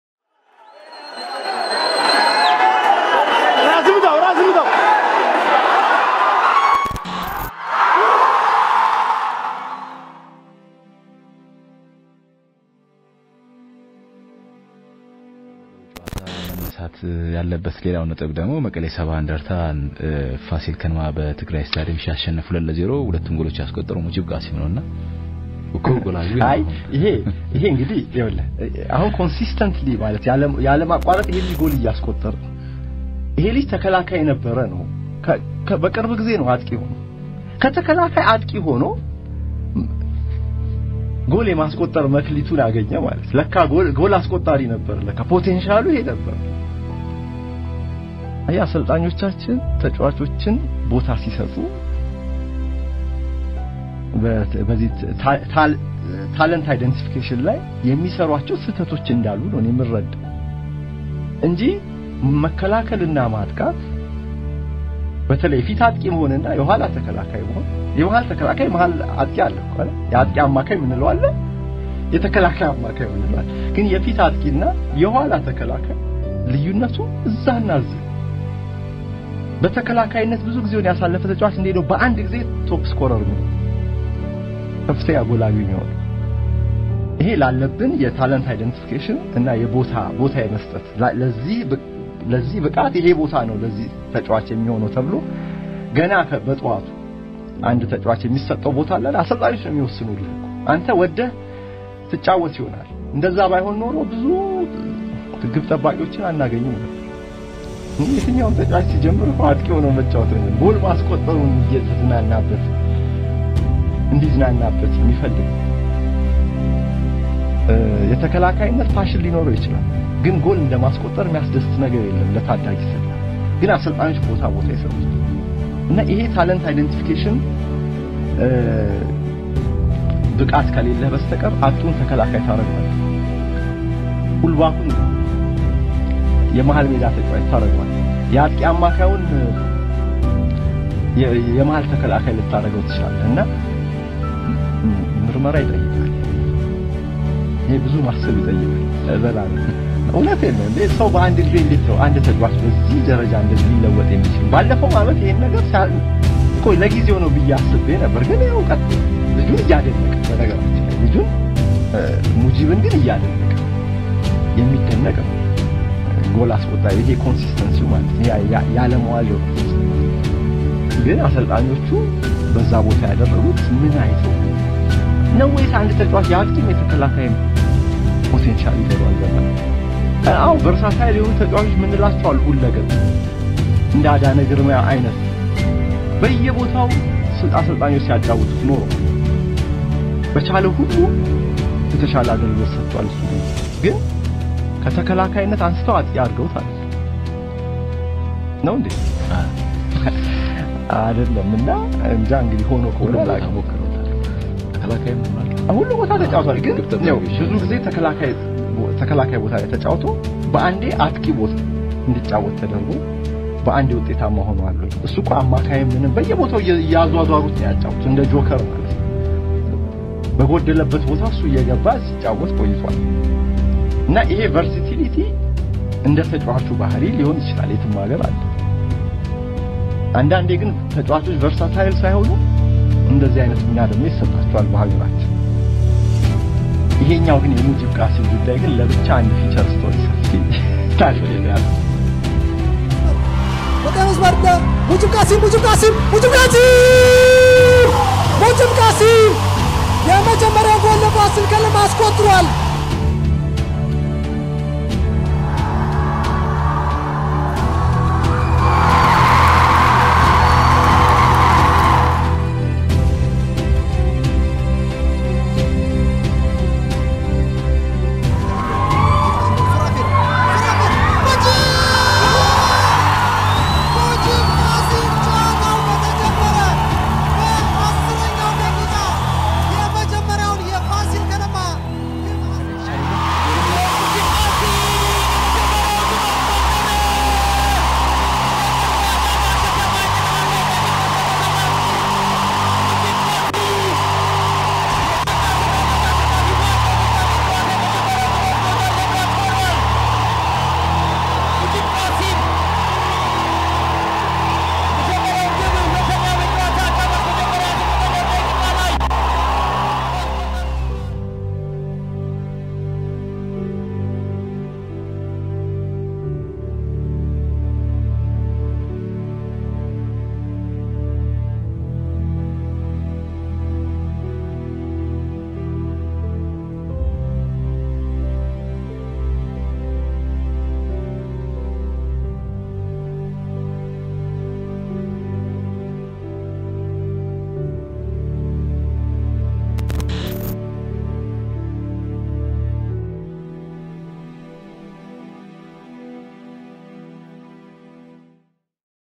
አስደናቂው የሙጂብ ቃሲም (Mujib Kasim) ለውጥ | "The Story"
የፋሲል ከነማው ሙጂብ ቃሲም አስደናቂ የቦታ ለውጥ እና ስኬት በመንሱር አብዱልቀኒ (Bisrat Sport 101.1) | Career Goals & Highlights (Edited by: Baloni Sport)

@ Baloni TV 2020